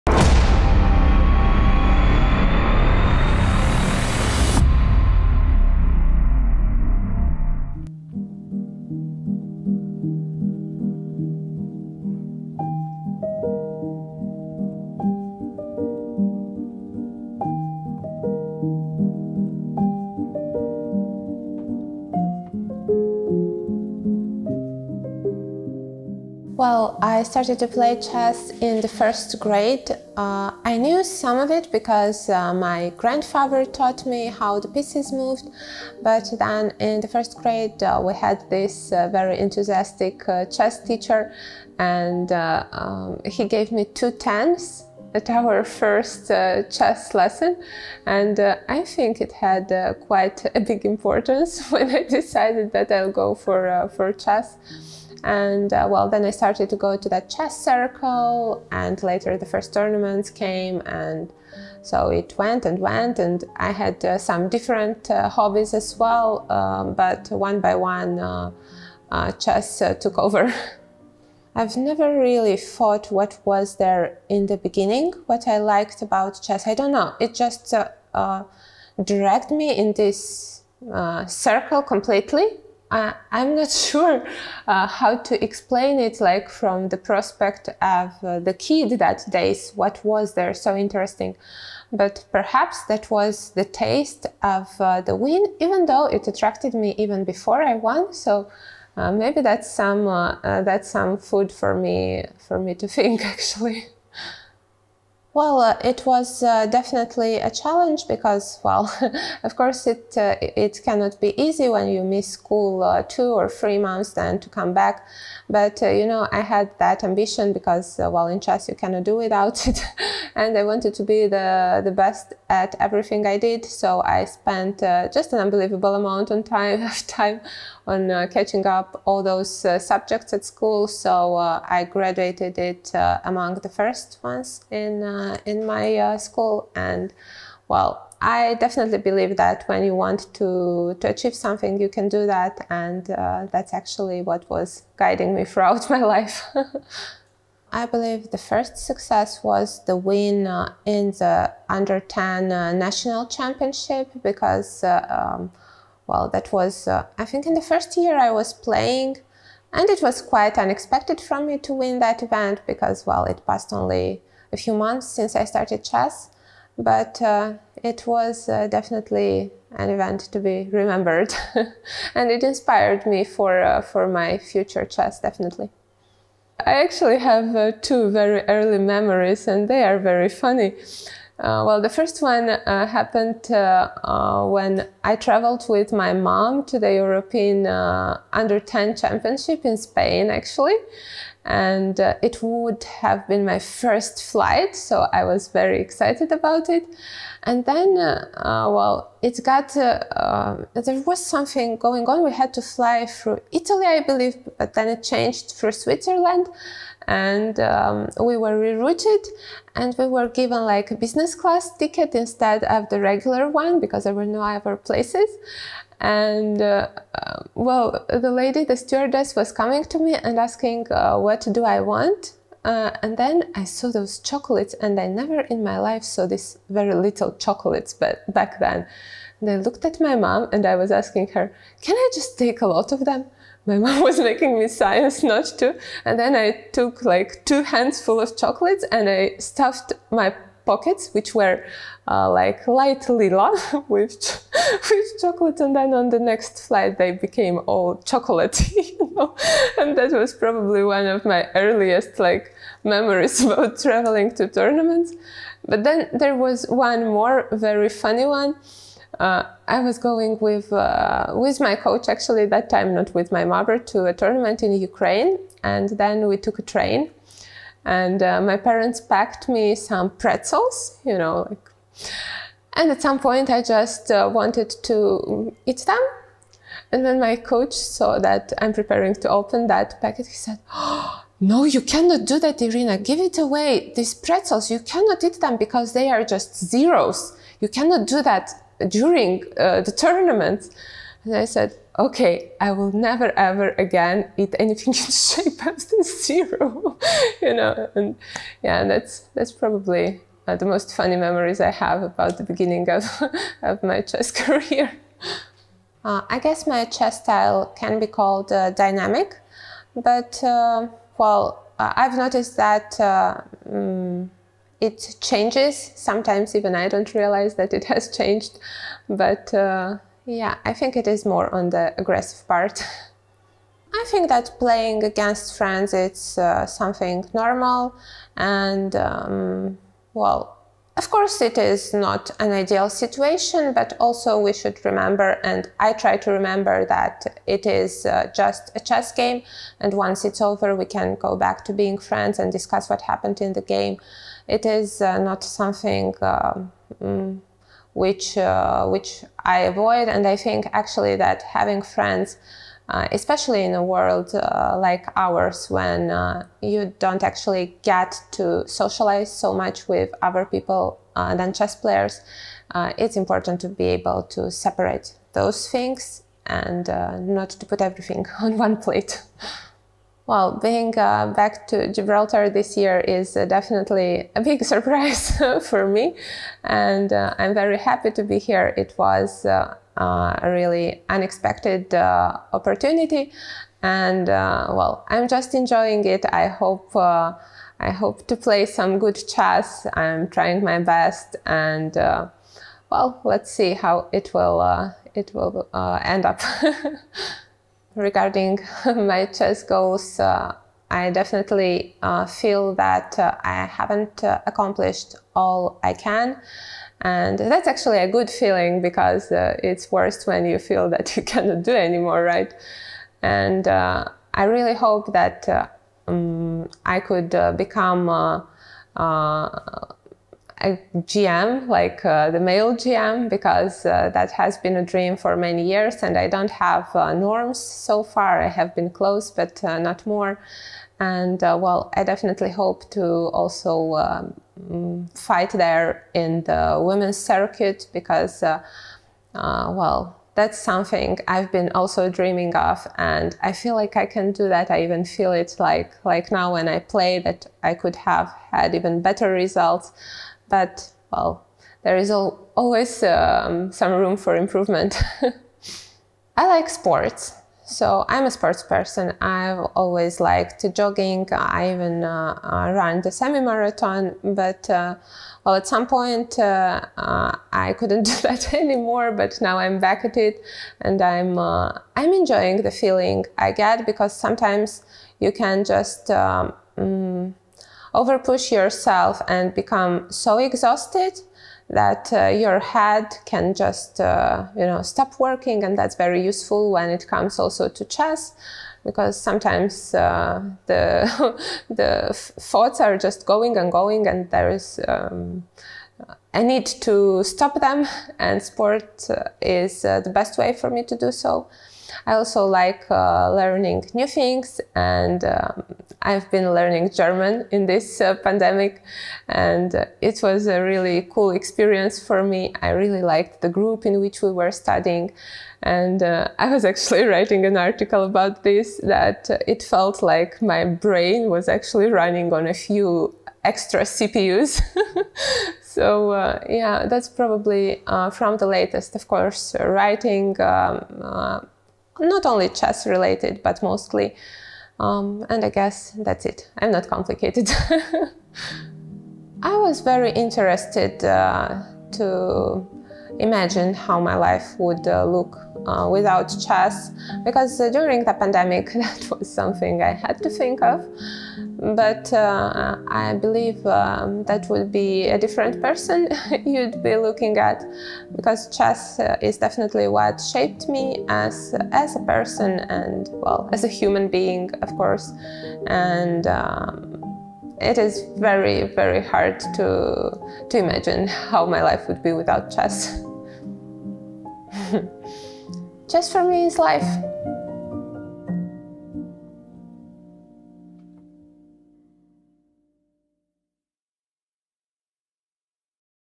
Meet Irina Bulmaga
In the series of interviews conducted at the Women's Grand Prix in Gibraltar, 2021, leading female chess players and female luminaries of the chess world talk about their life in chess, how they started, the difficulties they face, what the game means to them, and more.

The hero of this video, a Moldovan-born Romanian WGM Irina Bulmaga is the two-time Moldovan Women's Champion and a permanent member of the Romanian national team.